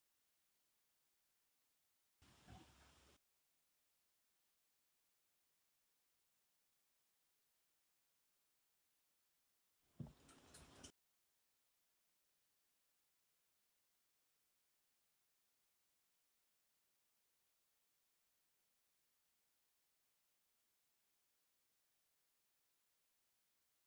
Tesla the Cat:- Flapping her big ears about!
Tess flapping her ears about in slow motion